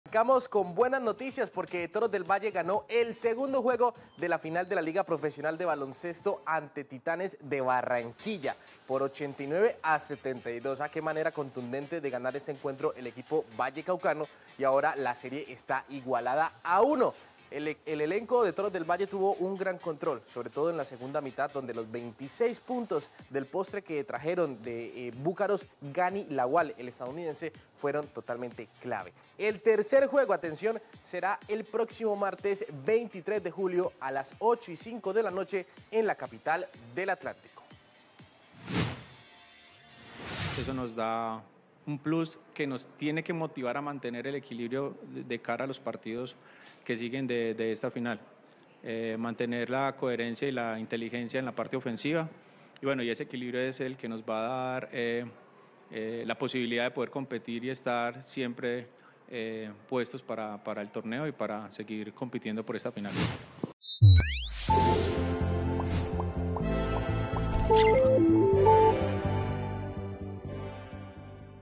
Toros del Valle ganó e igualó la serie ante Titanes de Barranquilla |21.07.2024| TP Noticias
Deportes | Toros del Valle ganó el segundo juego de la final de la Liga Profesional de Baloncesto a Titanes de Barranquilla, por 89 a 72, y ahora la serie está igualada a uno. El tercer juego será el próximo martes 23 de julio, a las 8:05 p.m. en la capital del Atlántico.